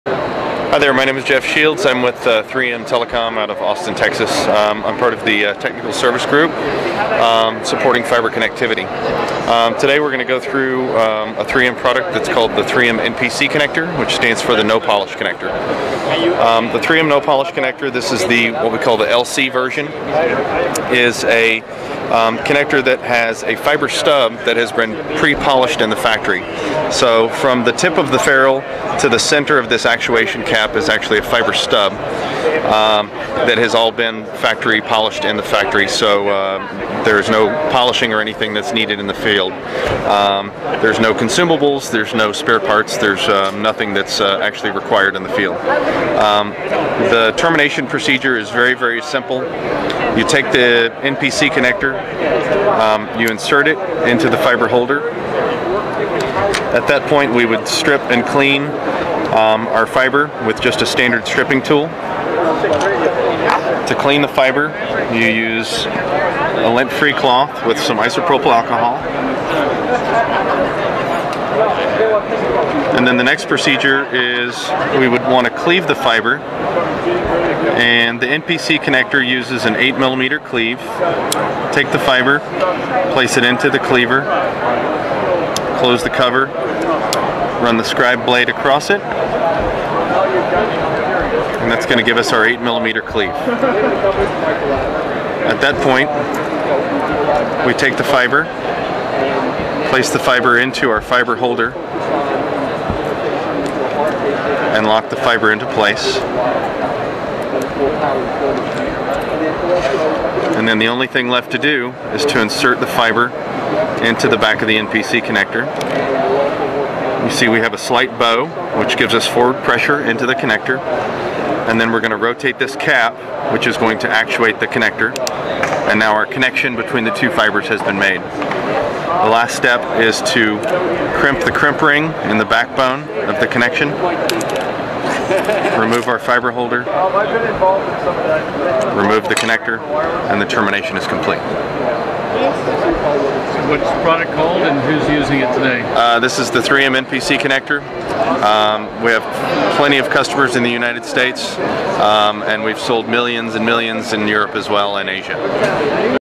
CTIA 2012: Who is afraid of splicing fiber when climbing towers?
Austin, TX based Jeff Shields, 3M Telecom Technical Service Group, provides RCR Wireless News an up close demonstration of a fiber connector (3M NPC no polish connector, LC version) termination procedure. Millions of the units have been sold in Europe, Asia and North America.